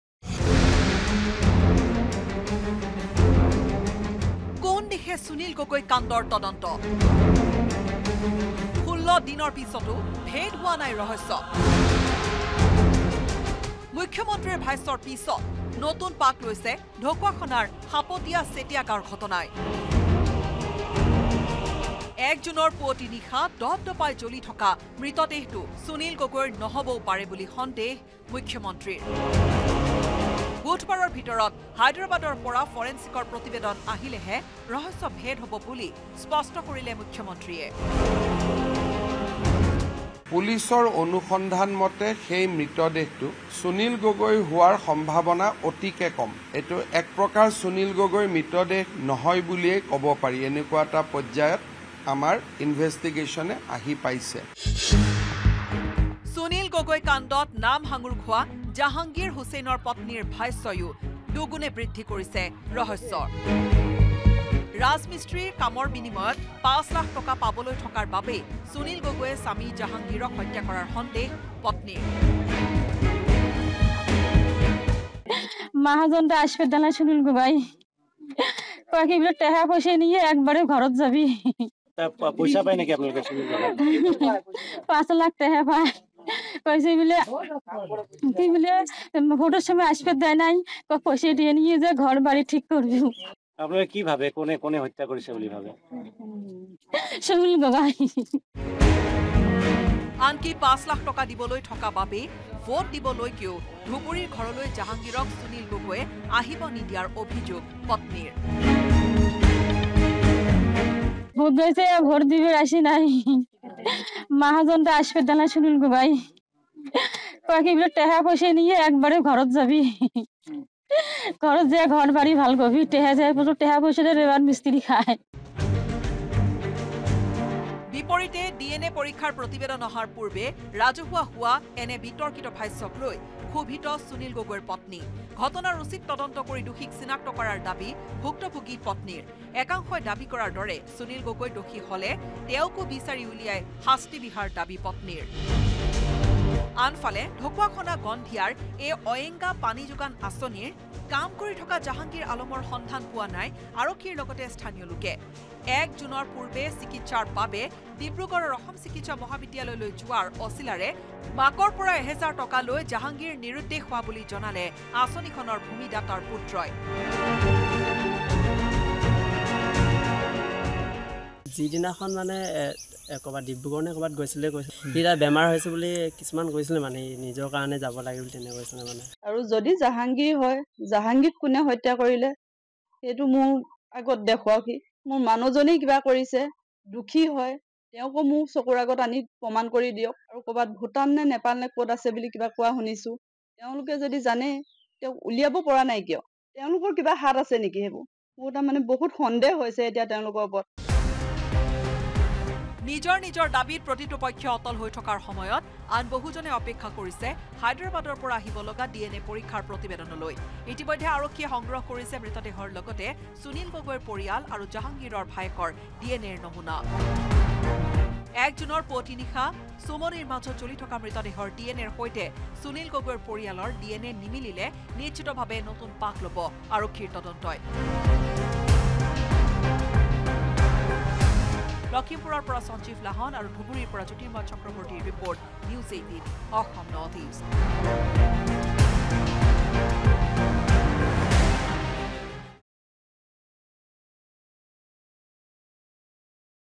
Sunil Gogoi Case Update : ৰহস্যঘন হৈ পৰিছে ঢকুৱাখনাৰ সুনীল গগৈ কাণ্ড | N18V
Sunil Gogoi Case Update | ৰহস্যঘন হৈ পৰিছে ঢকুৱাখনাৰ সুনীল গগৈ কাণ্ড। মুখ্যমন্ত্ৰীৰ ভাষ্যই ৰহস্যৰ মাজলৈ ঠেলি দিছে সমগ্ৰ ঘটনা। মৃতদেহটো সুনীল গগৈৰ নহ’বও পাৰে বুলি সন্দেহ কৰিছে মুখ্যমন্ত্ৰী হিমন্ত বিশ্ব শৰ্মাই। পকামিস্ত্ৰী জাহাংগীৰ হুছেইনৰ পত্নীৰ ভাষ্যই দুগুণে বৃদ্ধি কৰিছে ৰহস্য।